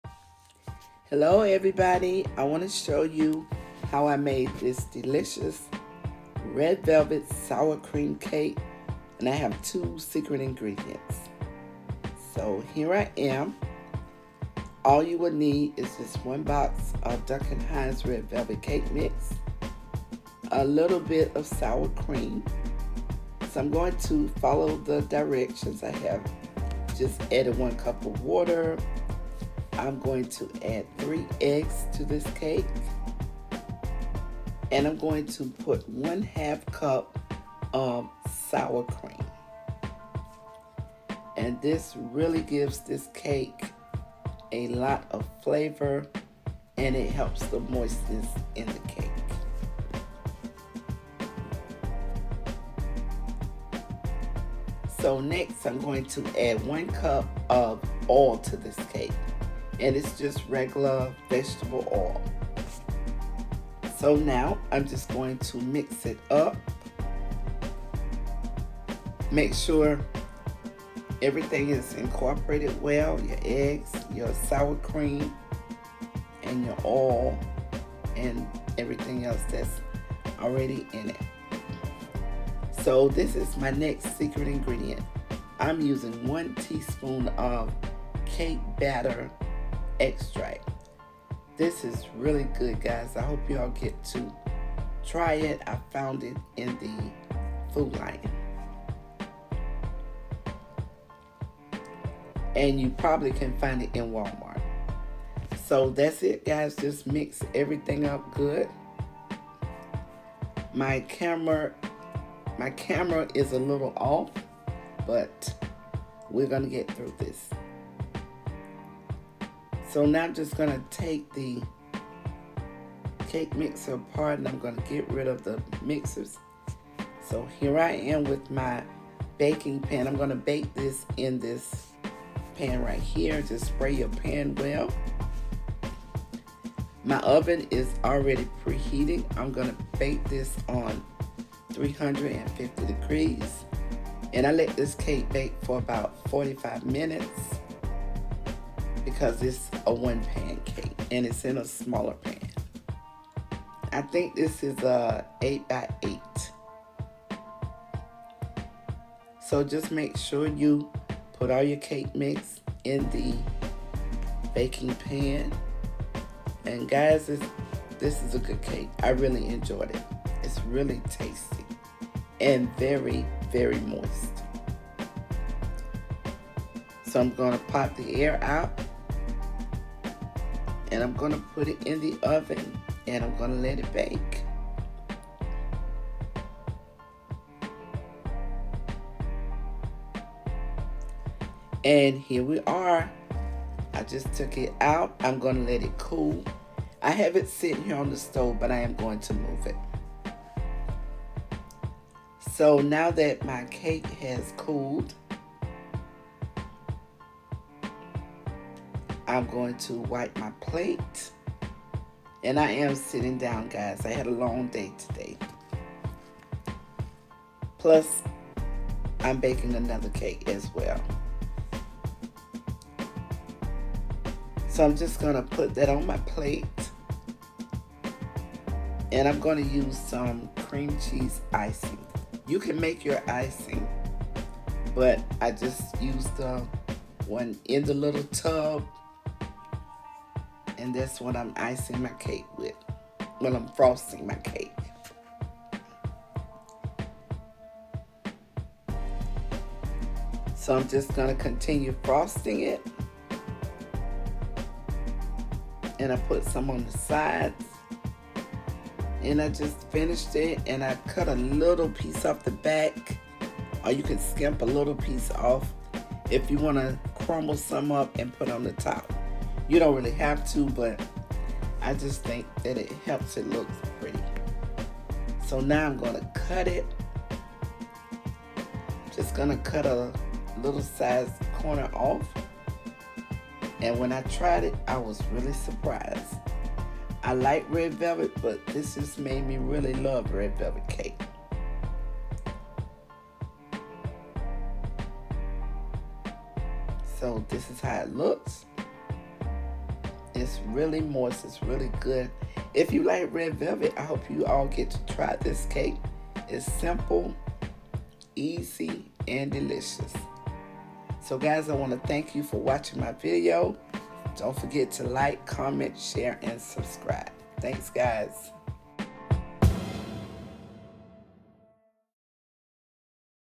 HOW TO MAKE A RED VELVET/SOUR CREAM LAYER CAKE| 2 Secret Ingredients| Delicious!
Follow the instructions on box and mix well. Next add 1/2 cup of sour cream and 1 teaspoon of Cake Batter extract and mix well. Preheat oven bake, cool and frost.